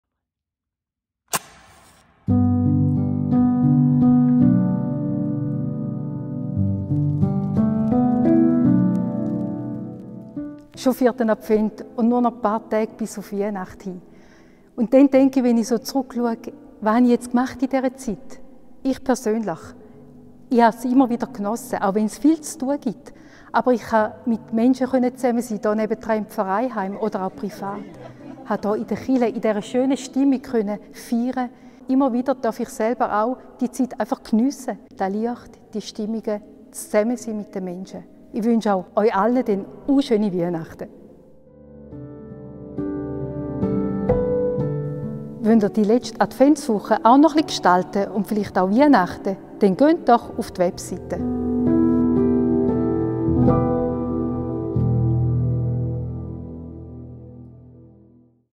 So bringst du Ruhe in den Advent - 4/4
4 St.Galler/innen erzählen. Mit Vreni Ammann, Seelsorgerin Rotmonten